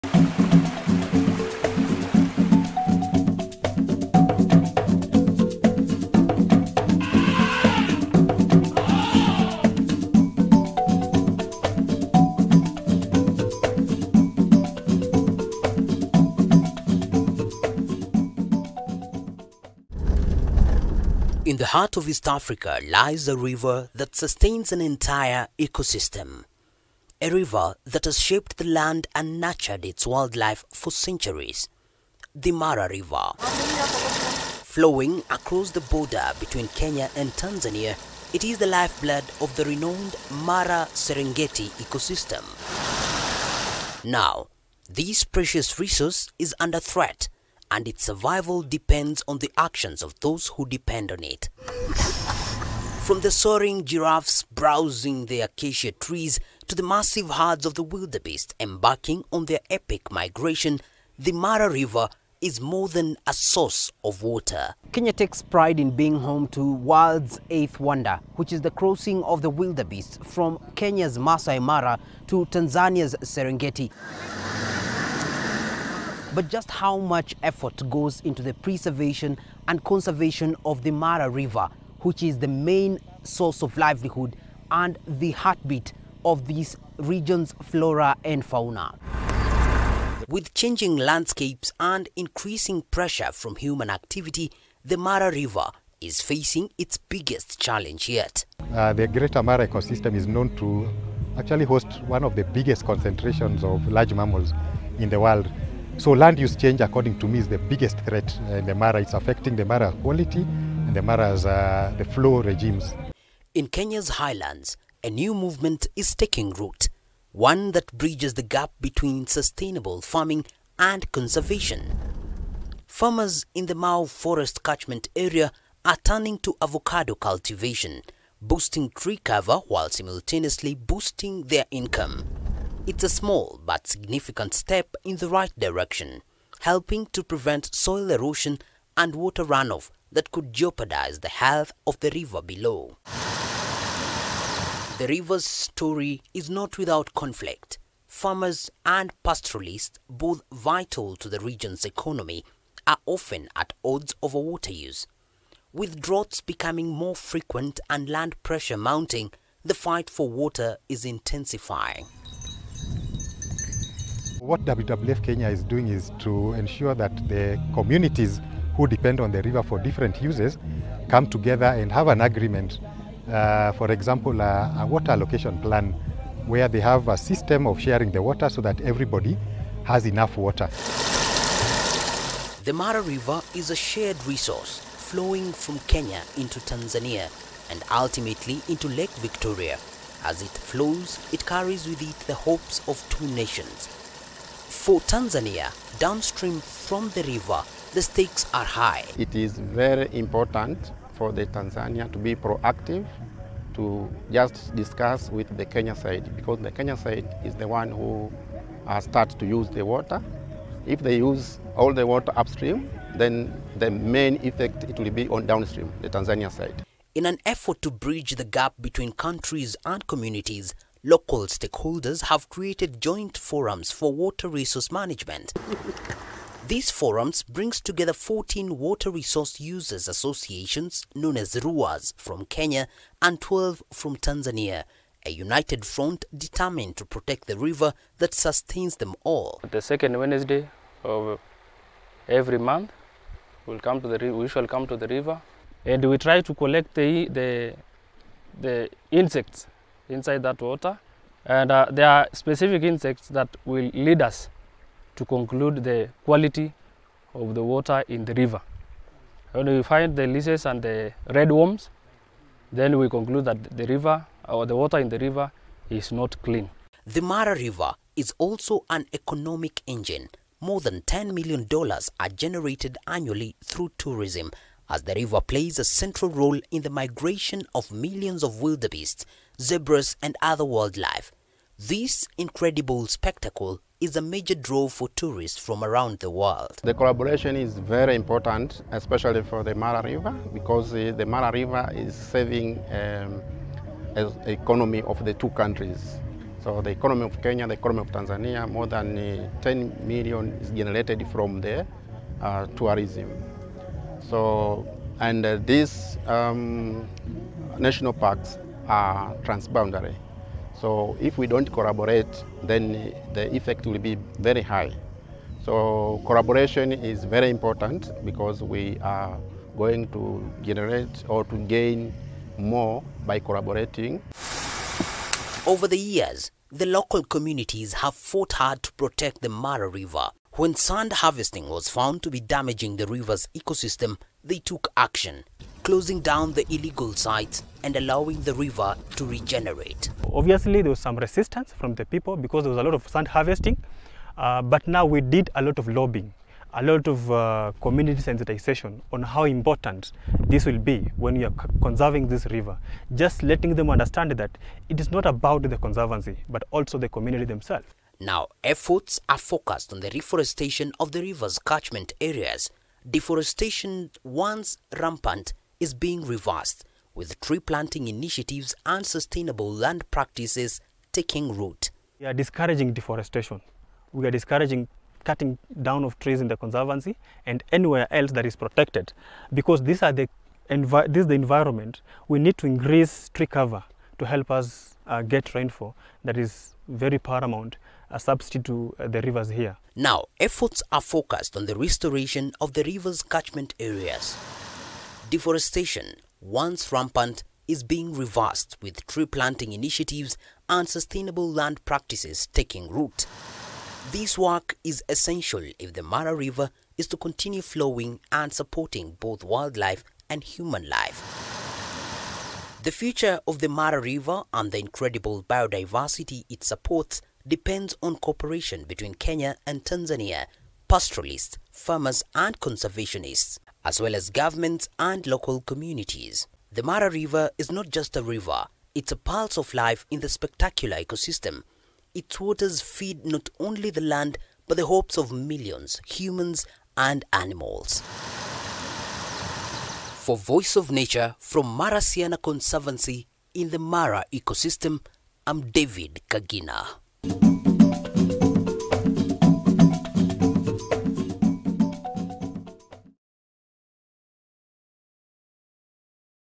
Kenya and Tanzania have to Collaborate to Save Mara River basin
In the heart of East Africa lies a river that sustains an entire ecosystem – a river that has shaped the land and nurtured its wildlife for centuries - the Mara River. flowing across the border between Kenya and Tanzania, it is the lifeblood of the renowned Mara-Serengeti ecosystem. Now, this precious resource is under threat, and its survival depends on the actions of those who depend on it. From the soaring giraffes browsing the acacia trees to the massive herds of wildebeest embarking on their epic migration, the Mara River is more than a source of water.